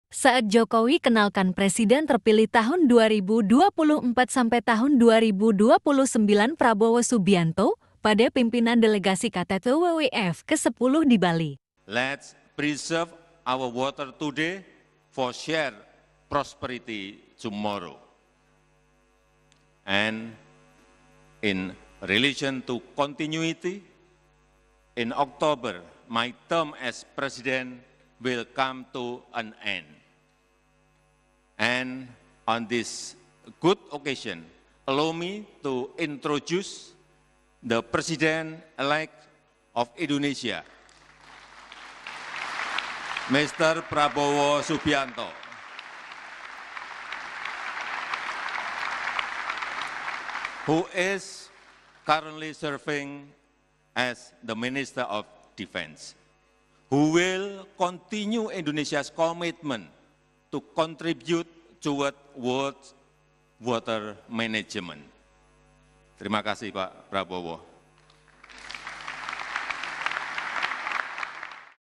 Jokowi perkenalkan Prabowo Subianto sebagai suksesor kepada delegasi WWF
Presiden Joko Widodo memperkenalkan Presiden Terpilih periode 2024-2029 Prabowo Subianto, kepada para delegasi World Water Forum (WWF) ke-10 di Bali.

Dalam sambutannya saat membuka rangkaian forum, Jokowi menyampaikan bahwa pada Oktober 2024 jabatannya sebagai Presiden akan berakhir.